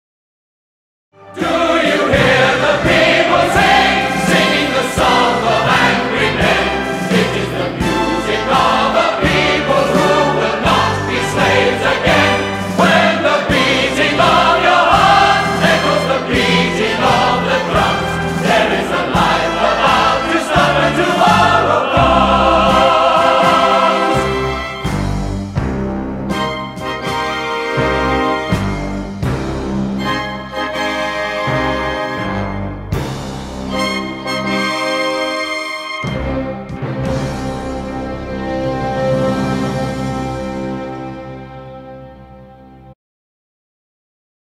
Musicals in Concert - Narvik Kulturhus
Prosjekt med 150 sangere, solister og musikere på scenen.
Arrangør: Norges Korforbund Produsent: Marita Havaas Olsen